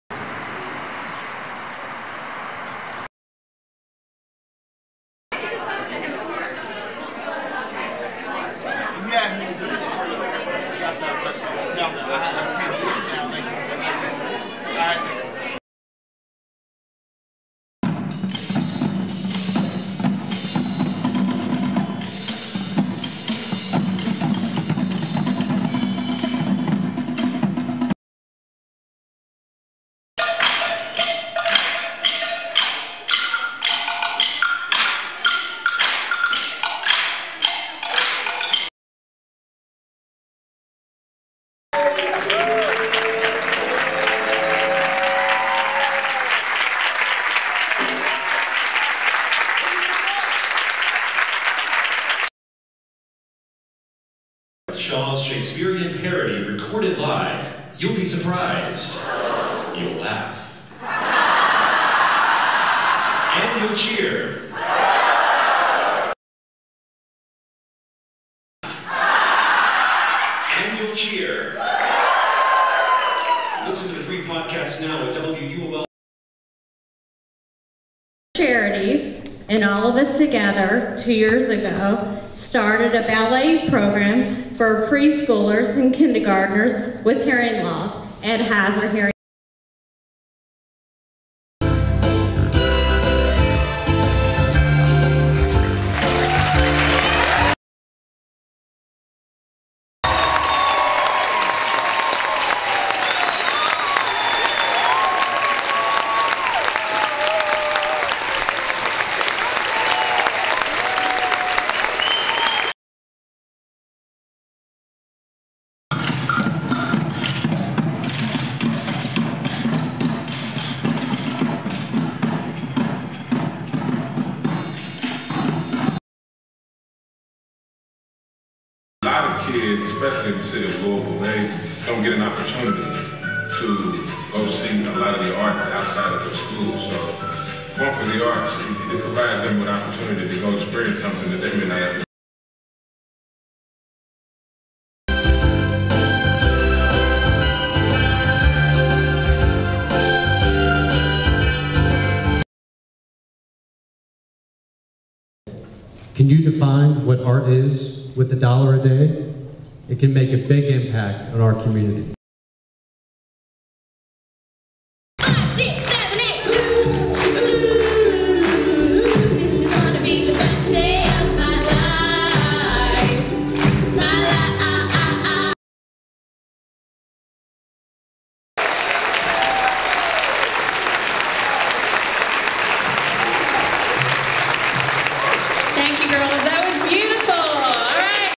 2015 Fund for the Arts Showcase & Campaign Kickoff Clip Reel
See clips from the 2015 Arts Showcase and Campaign Kickoff featuring performances by the River City Drum Corp, Kentucky Shakespeare, Kentucky Opera, Apalatin, Teddy Abrams, and more!